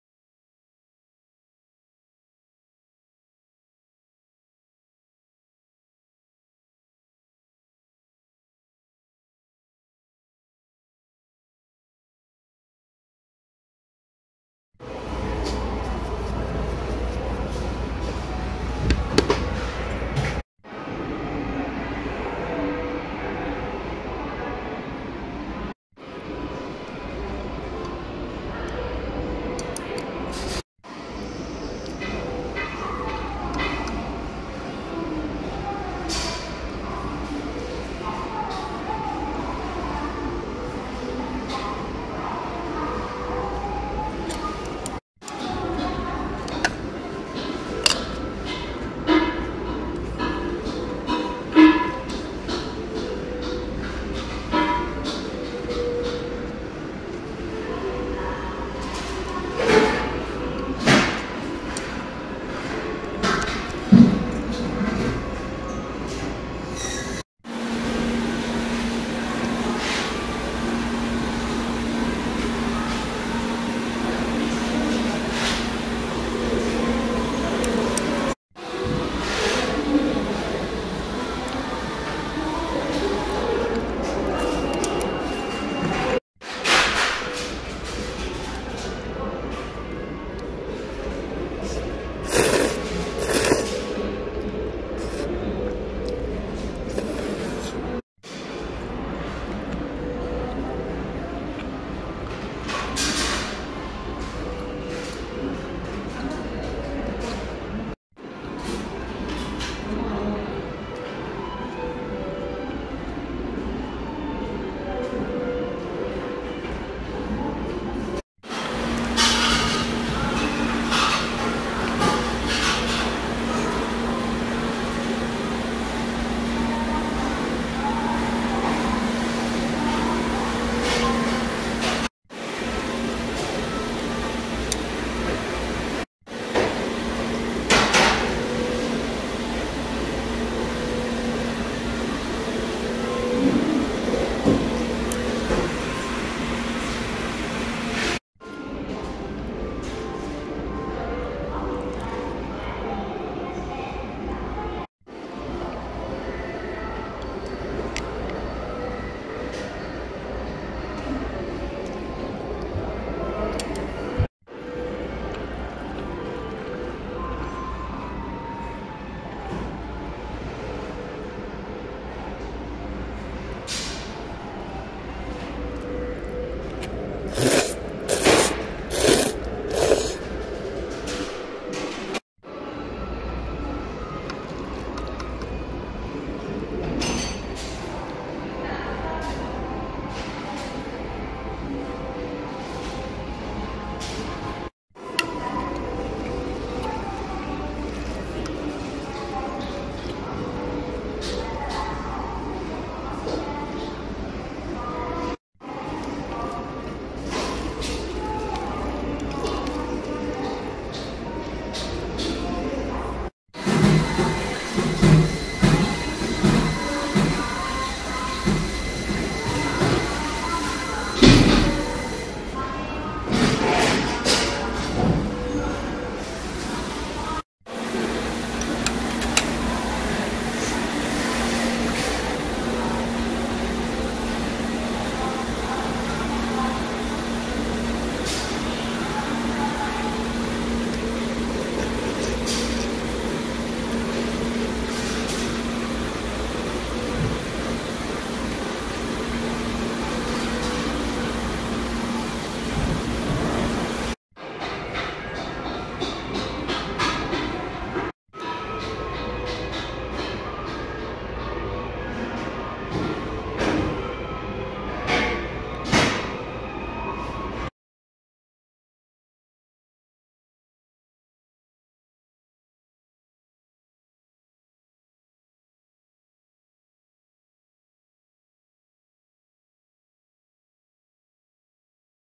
【スガキヤ】 4K 肉入りラーメン サラダセット 五目ごはん with Sony XPERIA1 Ⅱ
スガキヤ 肉入りラーメン サラダセット コショウは多めをお勧めします。
五目ごはんが美味しいです。
撮影使用端末 Sony Sony XPERIA1 Ⅱ Cinema Pro